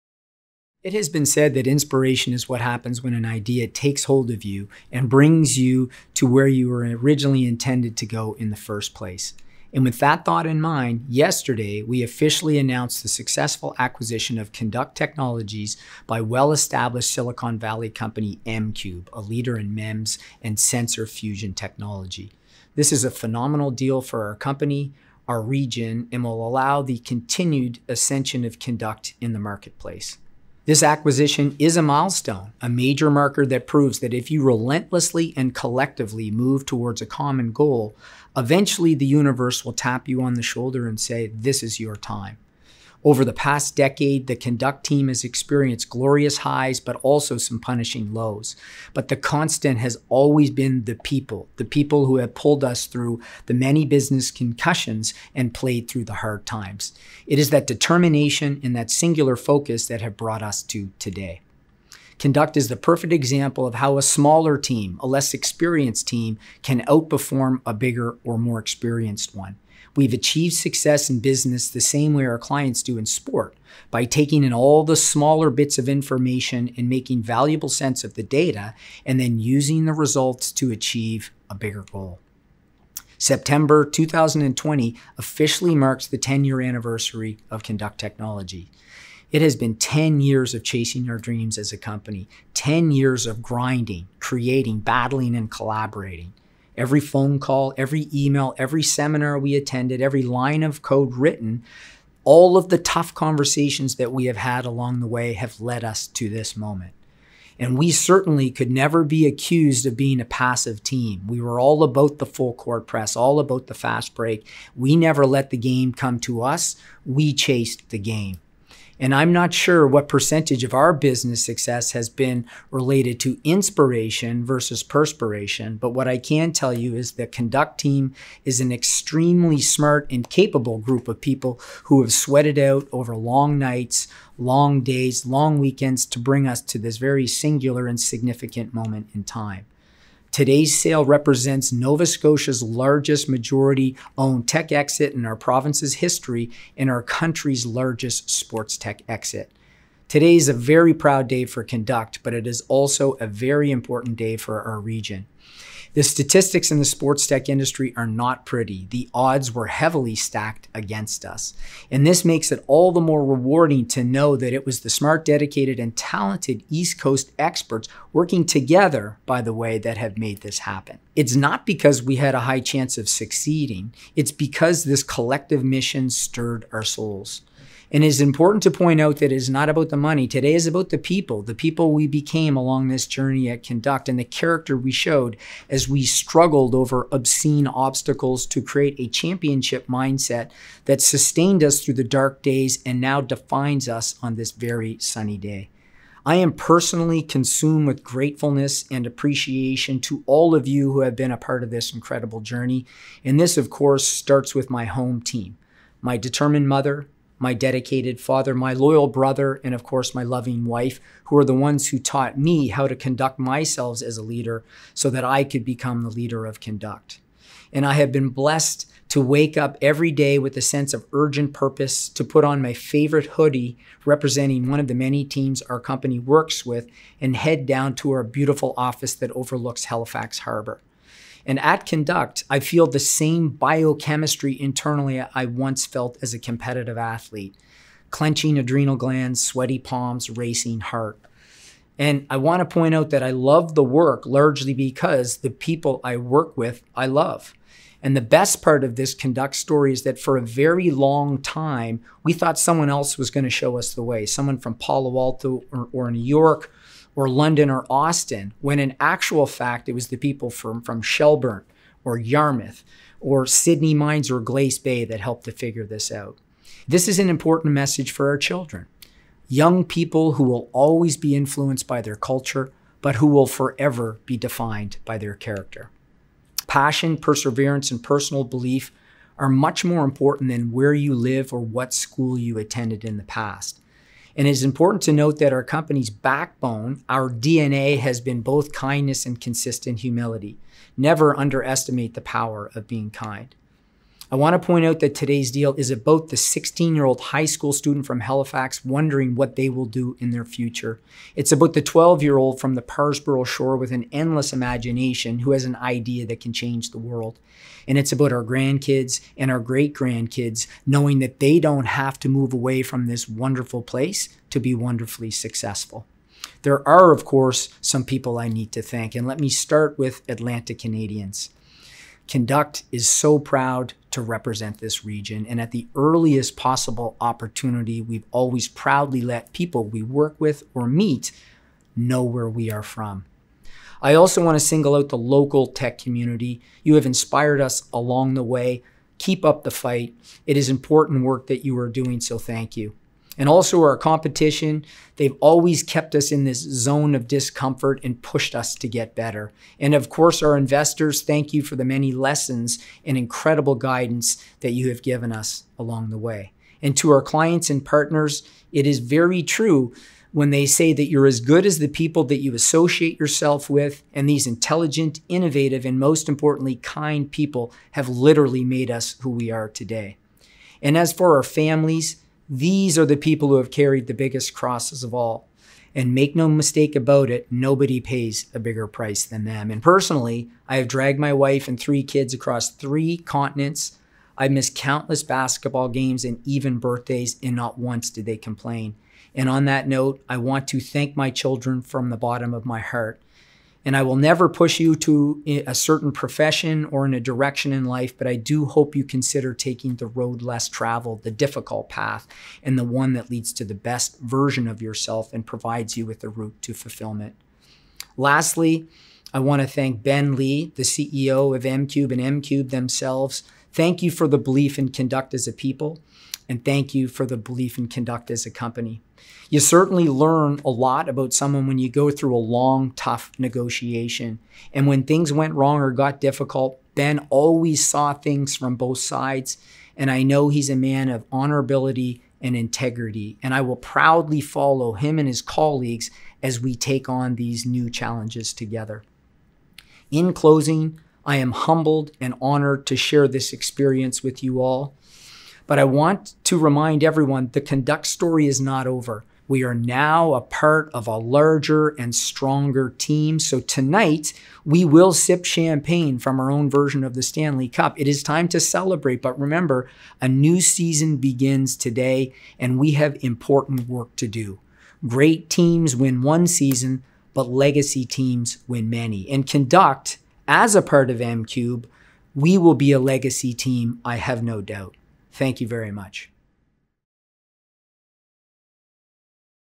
Kinduct Acquired by mCube - Thanks from our CEO, Travis McDonough
Kinduct is pleased to announce it has been acquired by mCube, the US-based company behind the smallest motion sensors in the world and a technology leader enabling the Internet of Moving Things (IoMT). The acquisition opens the door for Kinduct’s data consolidation and visualization engine to move beyond human performance into new biomechanical and digital health markets.